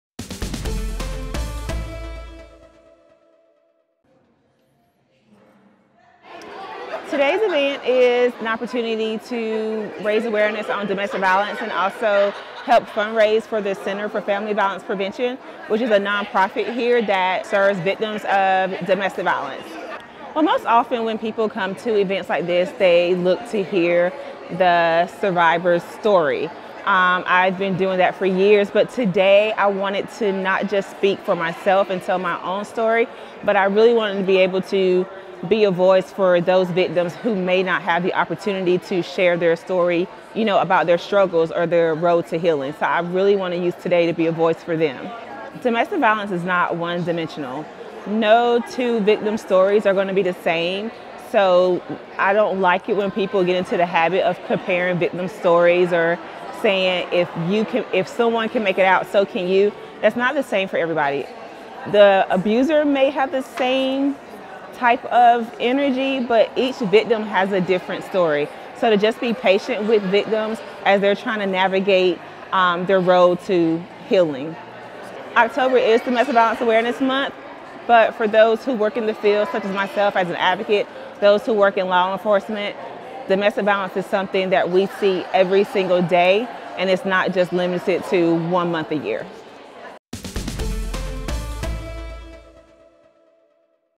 Local IMPACT Luncheon raises money to drive awareness of domestic violence
In the interview, the IMPACT Luncheon's special guest speaker and activist Deborah Sheppard talks about the luncheon, what it means to speak at this event and much more.